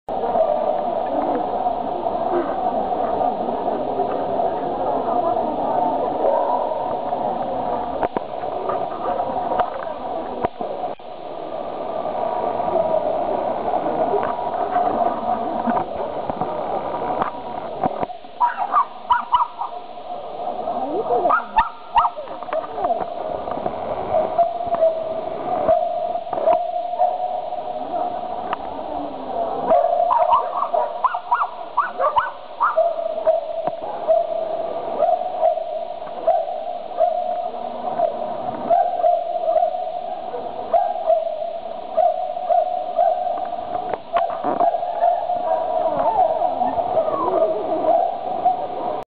Ruby - Jumping - Thames Valley UKA - 6/10/12
Ruby's first UKA show at Maidenhead on 6th October 2012 - this was her second run of the day - Jumping!